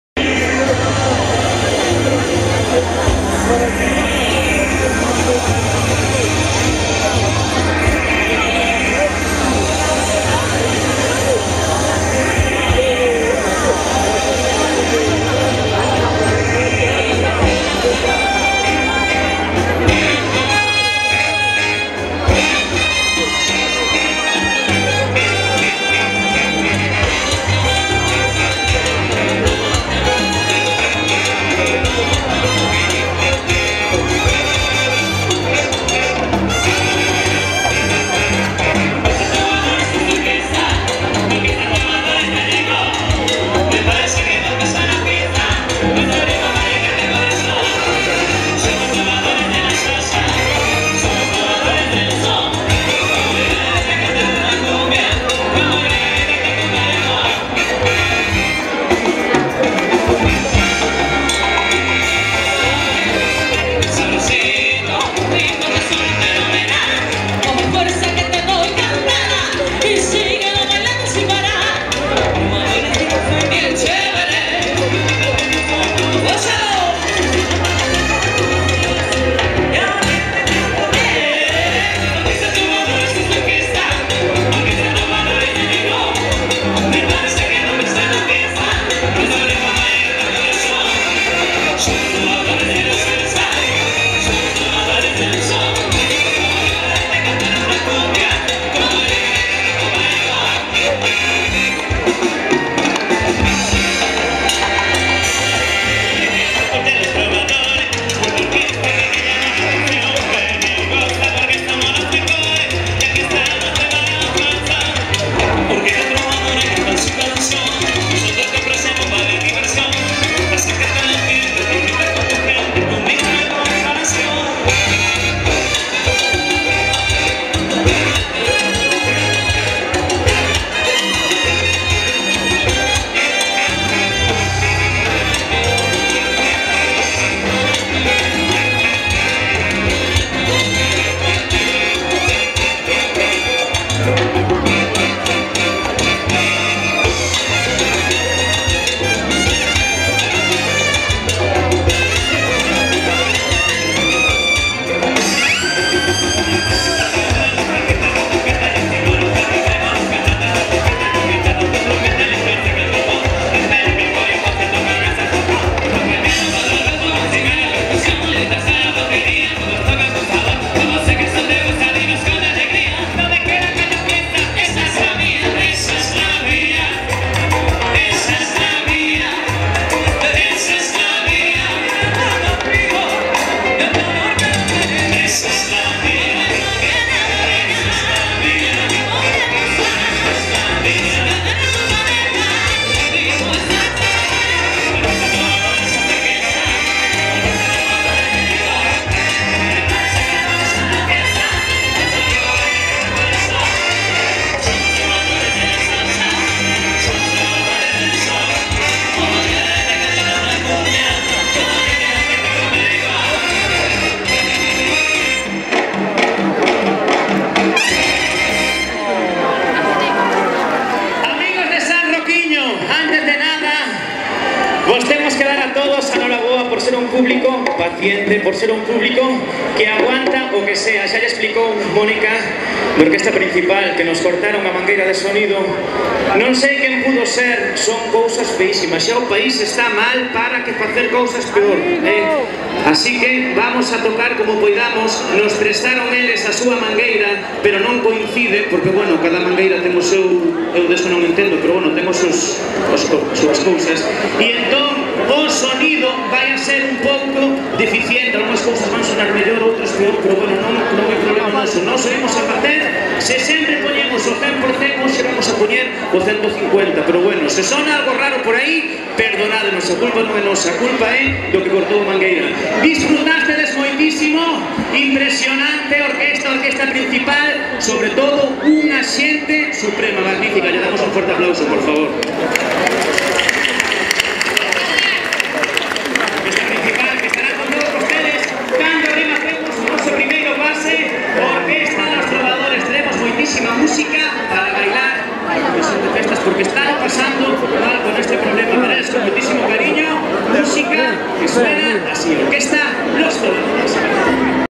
San Roquiño 2012 ~ Orquestra Los Trovadores
Actuación da Orquestra Los Trovadores, no derradeiro día das emblemáticas festas de San Roquiño de Tállara (Lousame).

Partillo este vídeo, como escoitaran con un son deficitario, porque como ben explica ao remate un dos vocalistas, sufriron unha sabotaxe no que lles cortaron a mangueira de son. Fago nada máis para que vexan os autores, o dano que lle puideron facer non só á orquestra, que xa llo fixeron, se non ao pobo de San Roquiño de Tállara e á comisión de festas.

Imaxínense que non houbese outra orquestra ou que os técnicos de son non fose capaces en menos de media hora, de facer compatible a conexión da orquestra Principal coa súa, teriámonos que marchar para a casa. Iso non se fai, porque polo xeral tódalas comisións, pero as de San Roquiño en especial, son singulares e non se merecían isto no colofón das súas festas.